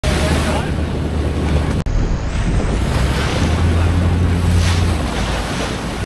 Black Marlin Breach Beside Boat
A Black marlin breaching the surface right next to the boat just before being tagged.